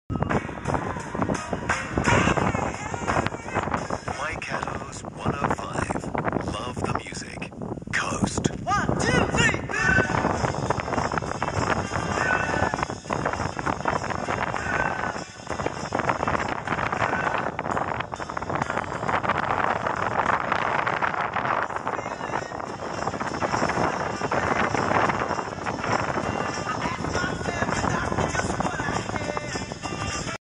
[T-R-O-P-O] 105 FM — THE COAST [HAMILTON]
Local Time: 14:33
Frequency: 105.00 MhZ
QTH: New Zealand, Nelson region, Coppermine Saddle, altitude: 878m)
Distance: 450 km
Ant.: telescopic ant
RX: SANGEAN ATS 909-X